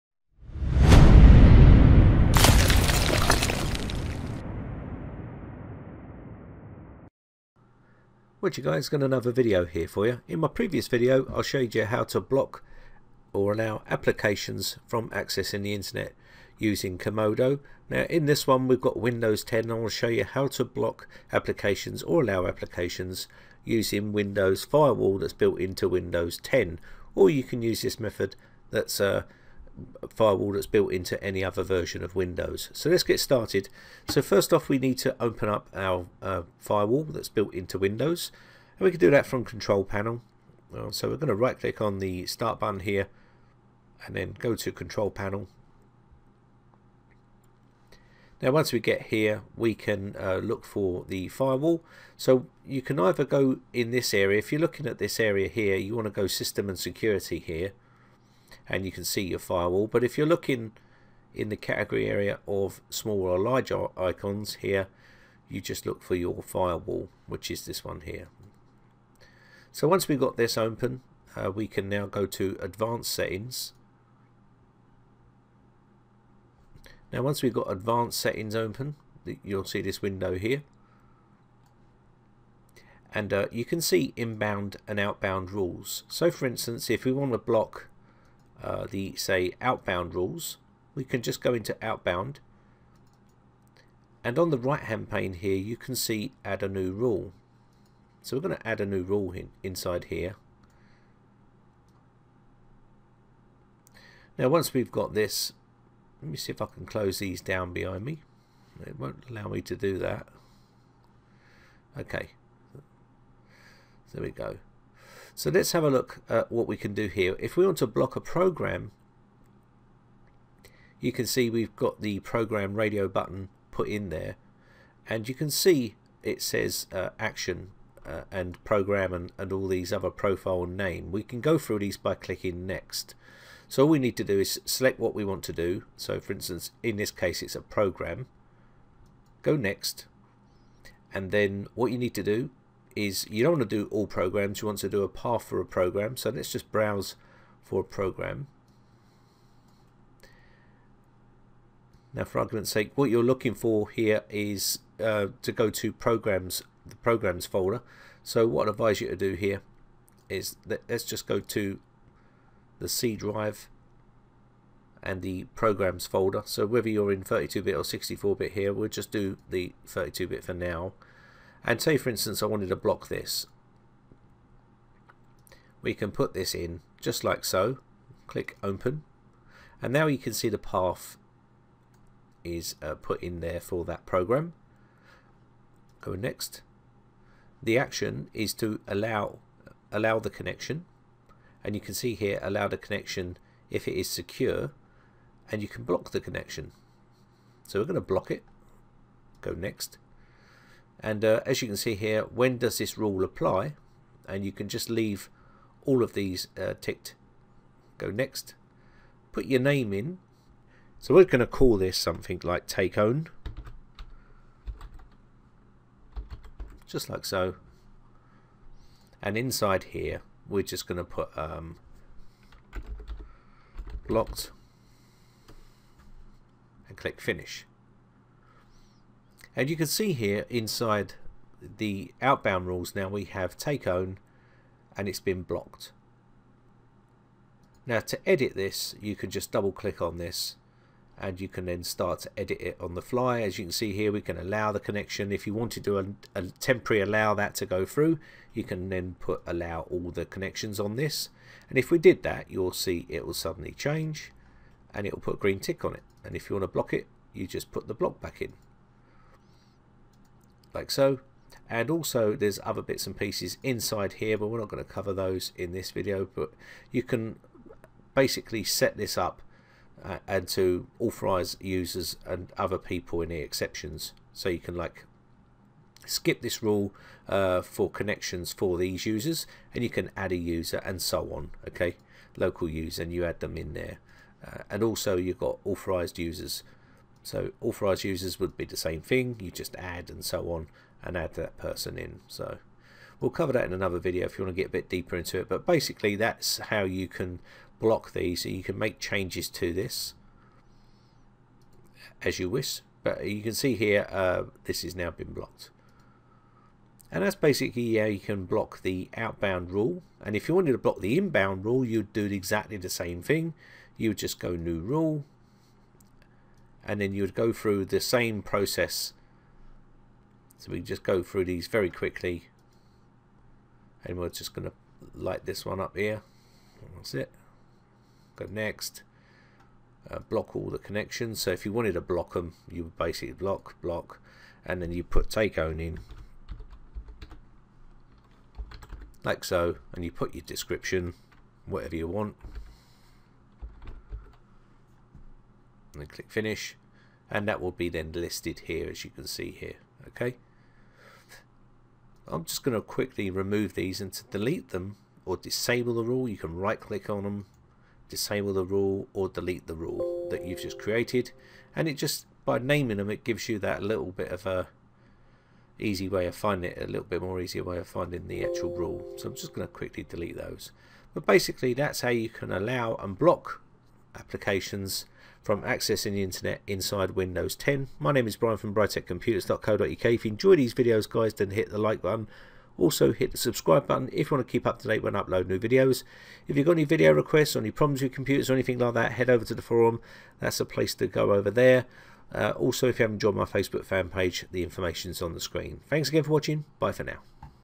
Block or Allow Applications Accessing Internet in Windows 10 Firewall
Windows built in firewall is a great way to control what comes in and goes out of your computer. Some programs want to access the internet for updates and other things, but some you might not want to update and you are happy to block internet access for them programs, you can even block a rogue process that is eating up your CPU by blocking its access to the internet.

if you need help with anything computer related, please join my forum.